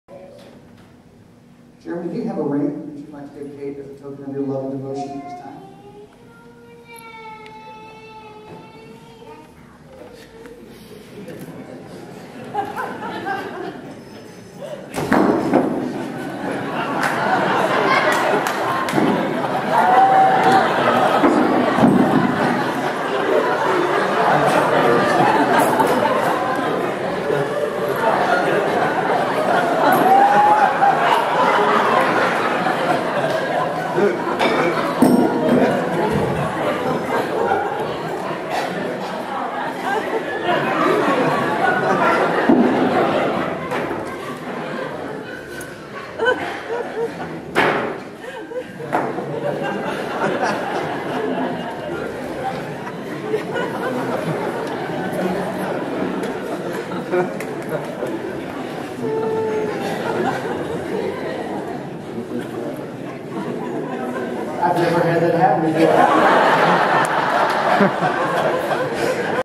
Wedding Prank Where's the Ring??
Our family and friends made our wedding perfect. My 4 best friends made it unforgettable. The pastor tops it off at the end.